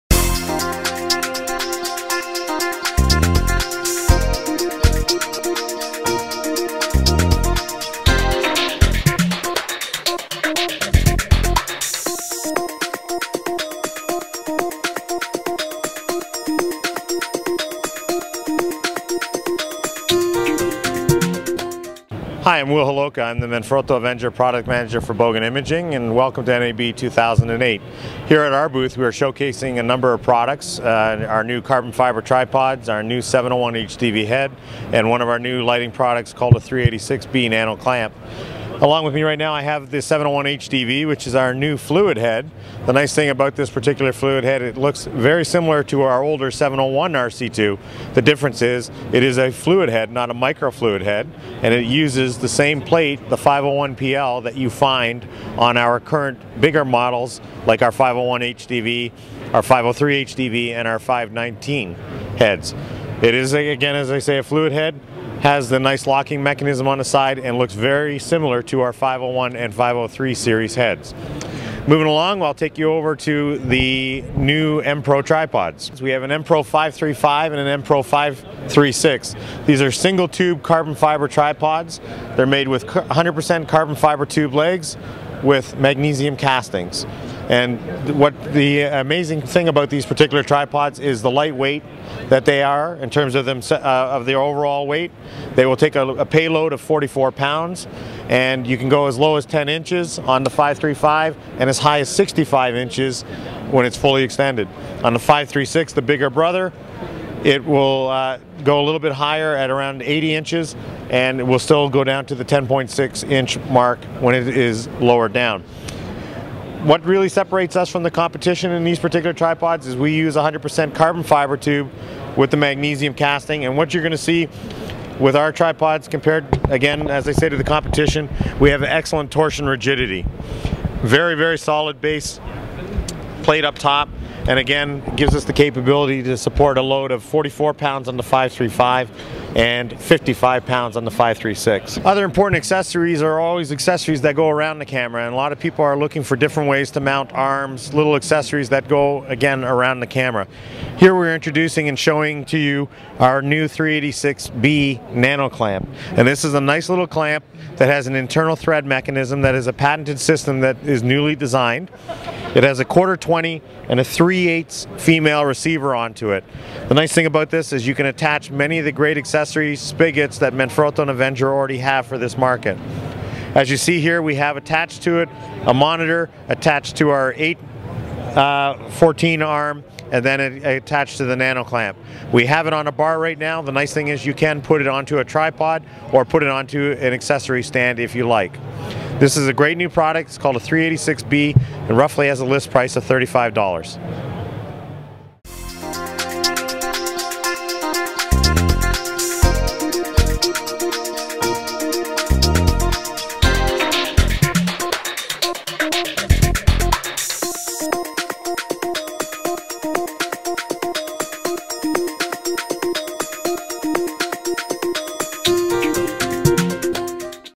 New Fluid Heads, Tripods & Clamps from Manfrotto
Markertek News Channel coverage of new products from Manfrotto at NAB 2008